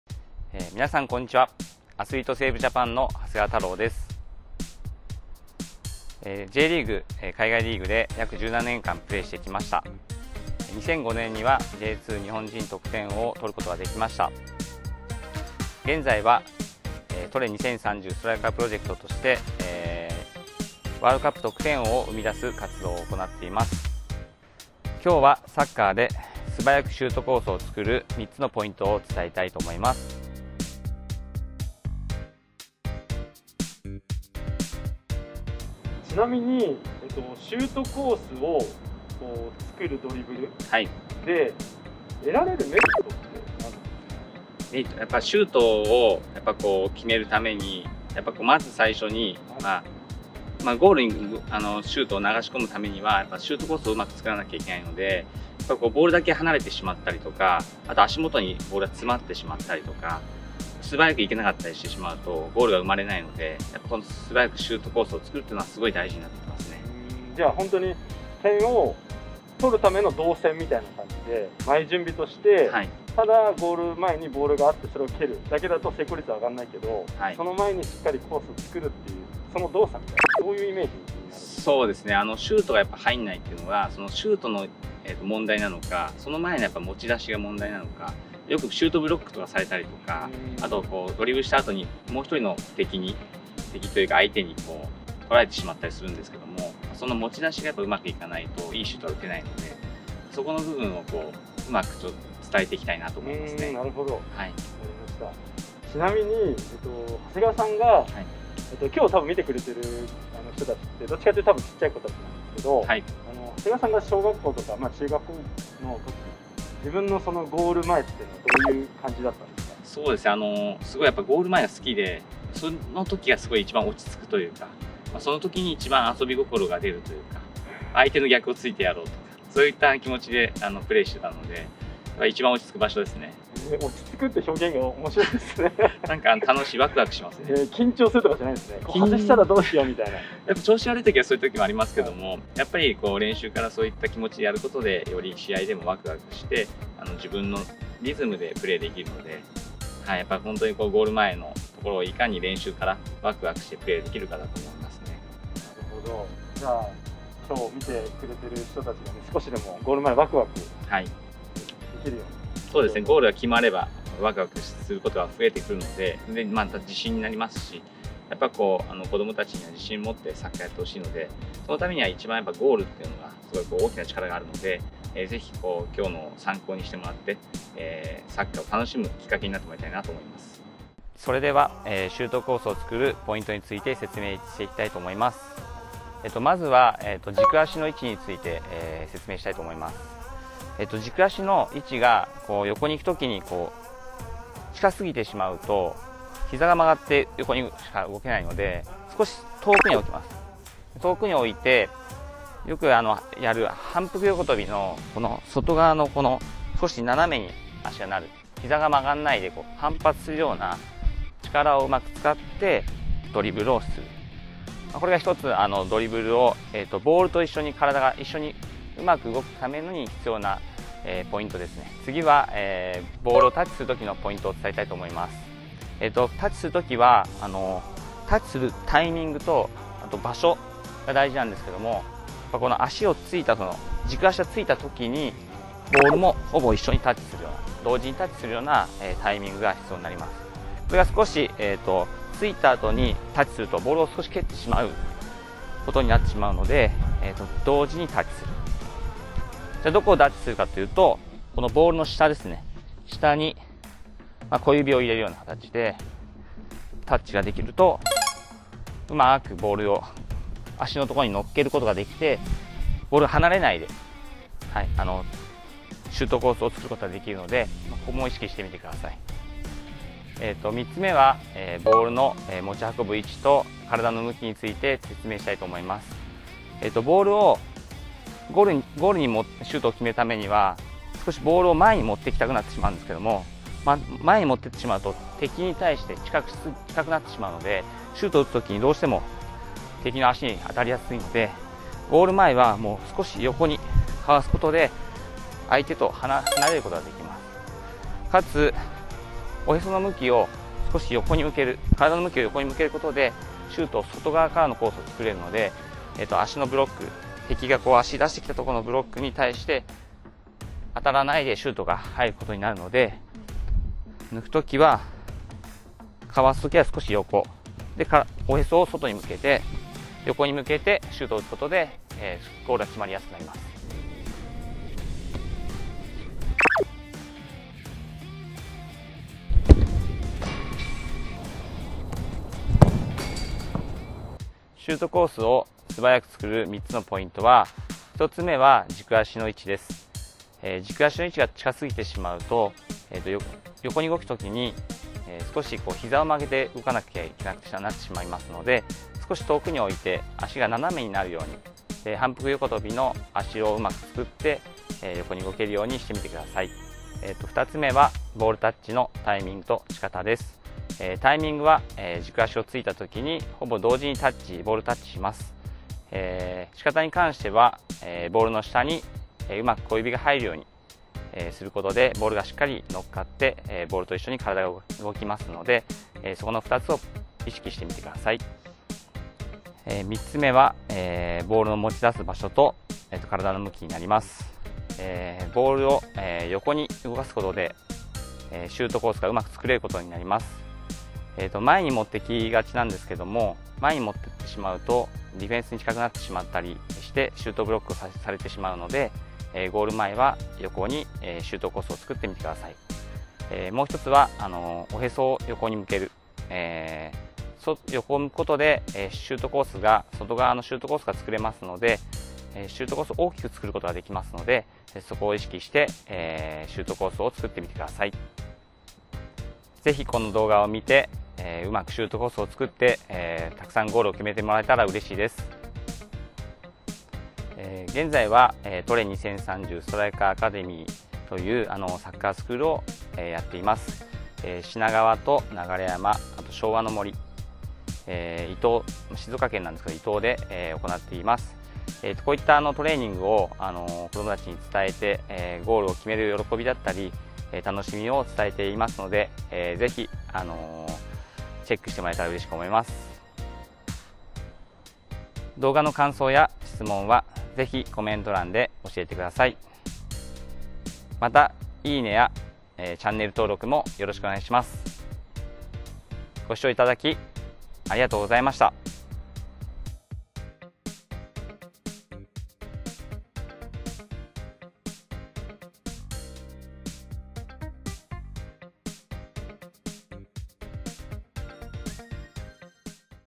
【サッカー】日本初ストライカーコーチが教える「ドリブルの極意」【スキルアップ】
【スキルアップ動画】
◆シュートに繋げるための「ドリブルの極意」

今回はASJアスリート 長谷川 太郎から、
シュートに繋げるためのドリブルの極意を教えてもらいます！

日本人初のストライカーコーチとして
どうすればシュートが決まるか、多くの人たちに
教え続けている元Jリーガー長谷川太郎さん。

ただディフェンスを抜くだけでなく、
どうすればより強く、防がれないシュートを打つ体勢を作れるか、
シュートコースをどう描くか、シュートを決めるための極意を伝授します。

この動画をみてより強いストライカーを目指そう！

【スーパースキルアップ！】シリーズとは...

元プロアスリートが教える、スポーツにおける身体能力や
スキルを向上させるための練習方法を紹介するスキルアップ動画シリーズ

―共同制作―

・株式会社ソーシャルキャピタル・プロダクション